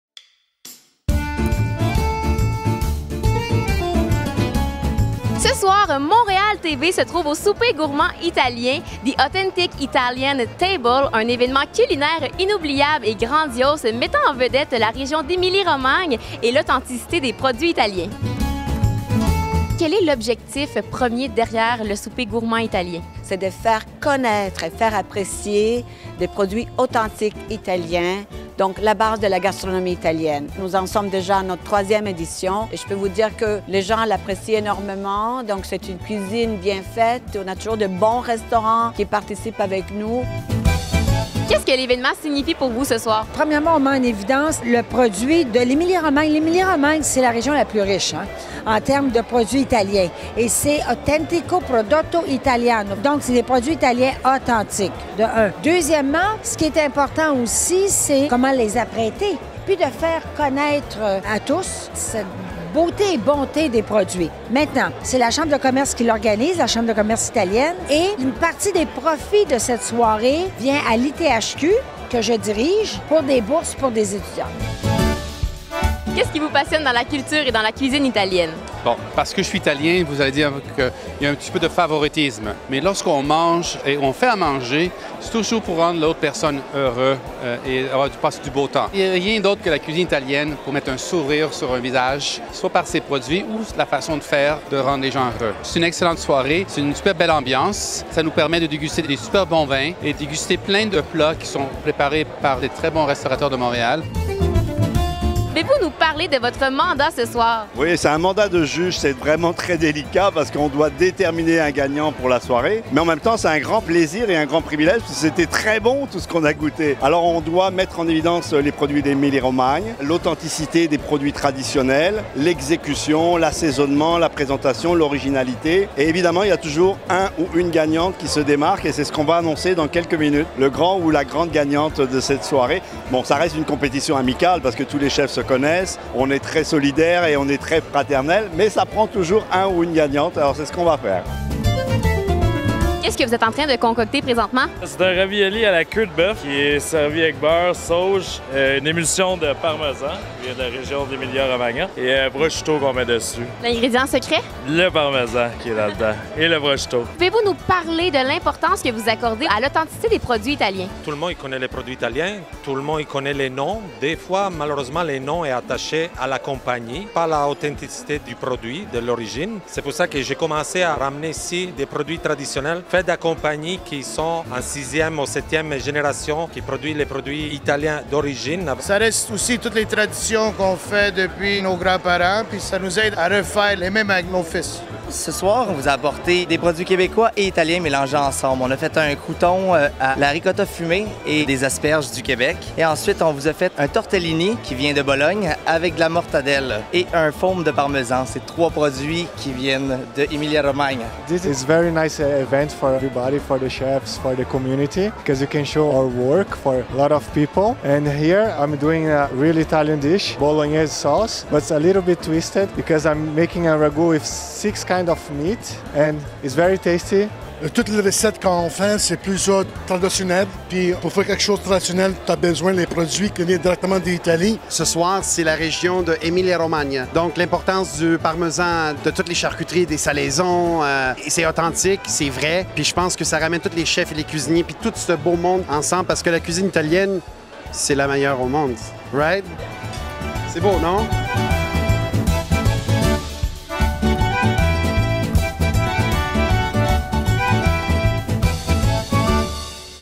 Délicieux Souper Gourmand Italien 2019 | Montreal.TV
Le Souper Gourmand Italien 2019 s'est déroulé au Grand Quai dans le Vieux-Port de Montréal.  Cette soirée met en vedette les excellents produits provenant de l'Italie.  L'événement est organisé par la Chambre de commerce italienne du Canada.